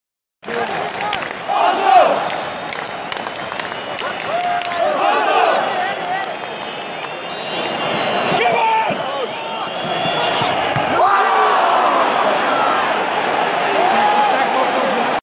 FutbalTour s exkluzivnym vyhladom na Emirates Stadium
FutbalTour na Emirates Stadium počas zápasu Ligy majstrov Arsenal - Marseille